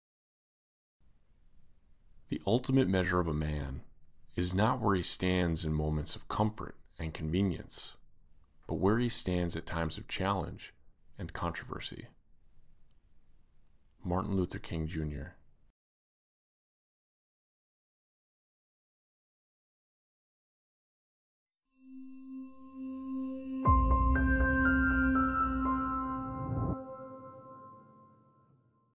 Martin Luther King Jr. Quote - The Measure of a Man!
This is a reading of a famous quote from Martin Luther King Jr.  The quote is: "The ultimate measure of a man is not where he stands in moments of comfort and convenience, but where he stands at times of challenge and controversy."